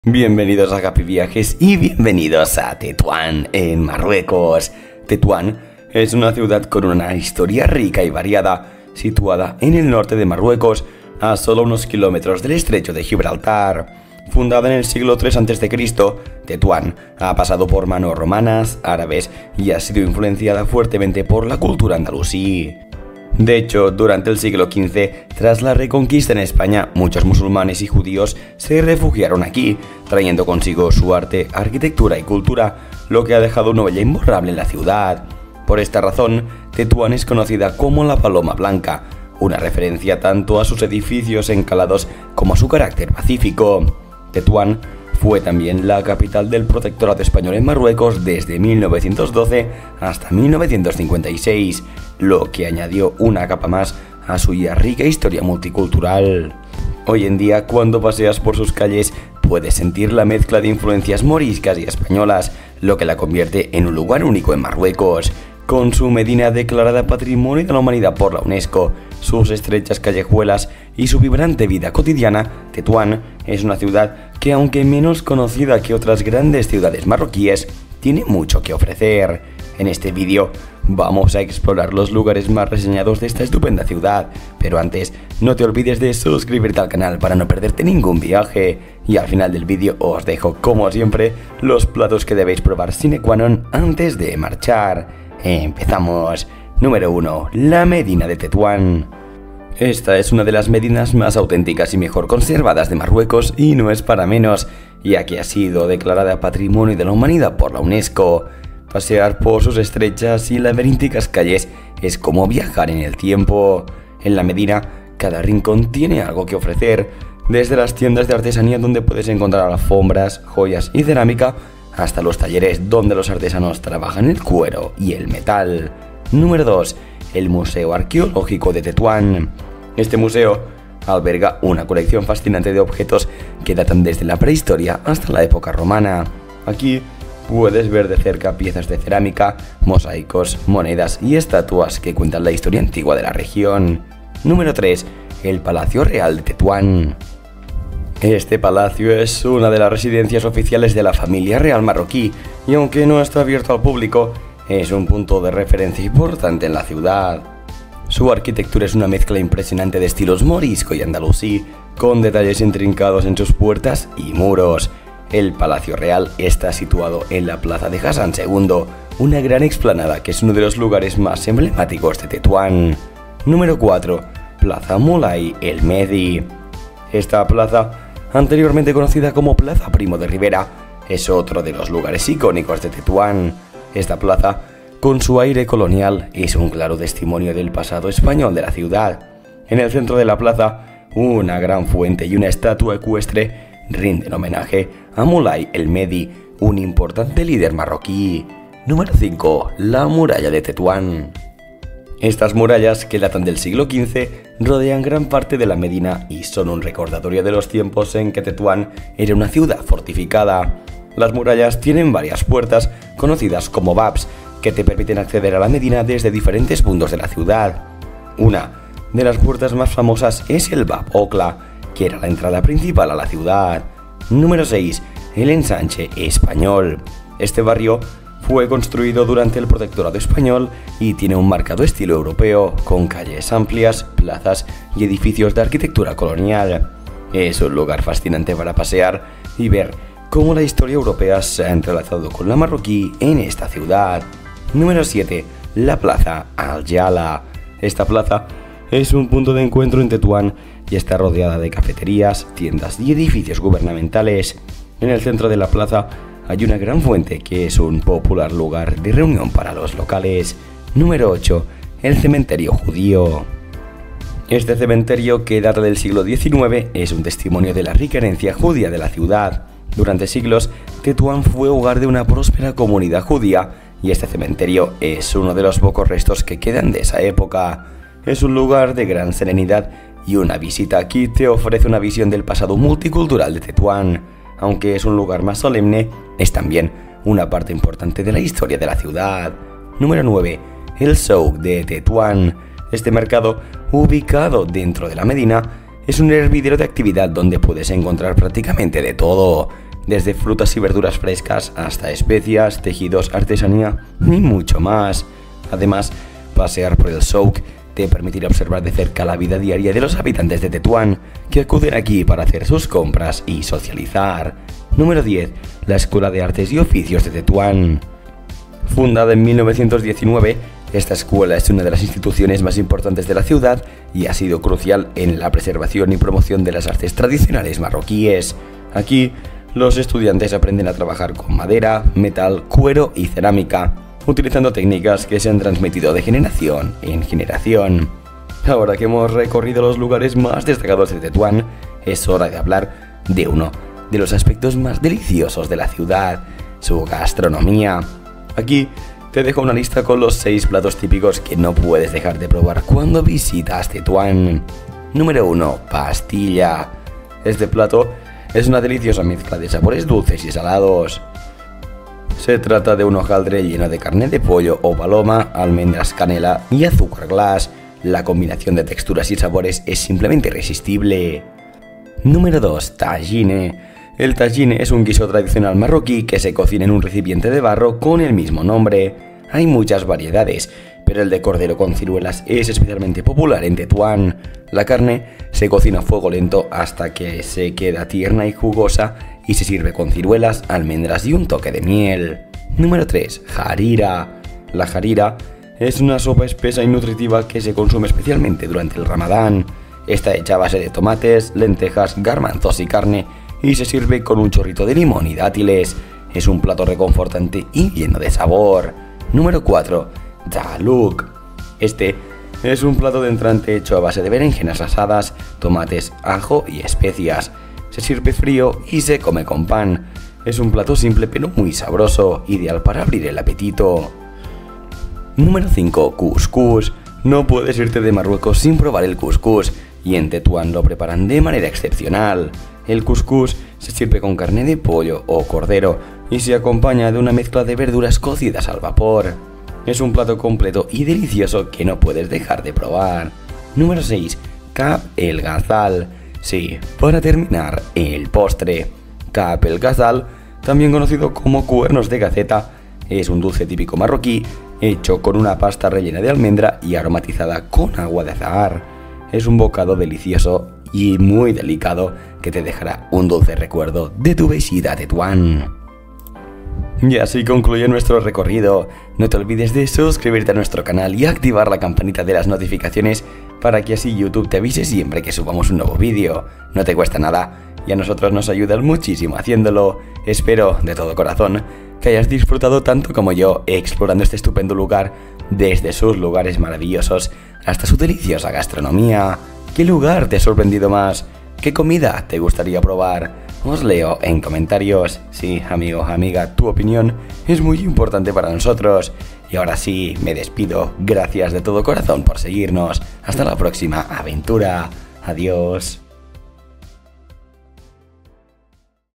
✈️✈️TETUAN | MARRUECOS | Historia | Lugares IMPRESCINDIBLES | CURIOSIDADES | Qué COMER😮😮
¿Planeando un viaje a TETUAN, EN MARRUECOS ???

Descúbrela con HAPPY VIAJES!! No te pierdas nuestros viajes,

Tetuán es una ciudad con una historia rica y variada, situada en el norte de Marruecos, a solo unos kilómetros del estrecho de Gibraltar. Fundada en el siglo III a.C., Tetuán ha pasado por manos romanas, árabes, y ha sido influenciada fuertemente por la cultura andalusí. De hecho, durante el siglo XV, tras la Reconquista en España, muchos musulmanes y judíos se refugiaron aquí, trayendo consigo su arte, arquitectura y cultura, lo que ha dejado una huella imborrable en la ciudad. Por esta razón, Tetuán es conocida como "La Paloma Blanca", una referencia tanto a sus edificios encalados como a su carácter pacífico.
Tetuán fue también la capital del Protectorado Español en Marruecos desde 1912 hasta 1956, lo que añadió una capa más a su ya rica historia multicultural. Hoy en día, cuando paseas por sus calles, puedes sentir la mezcla de influencias moriscas y españolas, lo que la convierte en un lugar único en Marruecos. Con su Medina declarada Patrimonio de la Humanidad por la UNESCO, sus estrechas callejuelas y su vibrante vida cotidiana, Tetuán es una ciudad que, aunque menos conocida que otras grandes ciudades marroquíes, tiene mucho que ofrecer.

 y al final del video os dejo como siempre los platos que debéis probar sine qua non antes de marchar!!

EMPEZAMOSSSS

Aquí os dejo el guión:

00:00 INTRODUCCION
01:20 La Medina de Tetuán
02:05  El Museo Arqueológico de Tetuán
02:25 El Palacio Real de Tetuán
02:55 Plaza Moulay El Mehdi
03:20 La Muralla de Tetuán
03:45 El Ensanche Español
04:05 La Plaza Al-Jalaa
04:25 El Cementerio Judío
04:50 El Souk de Tetuán
05:15 La Escuela de Artes y Oficios de Tetuán
05:55 Comida típica TETUAN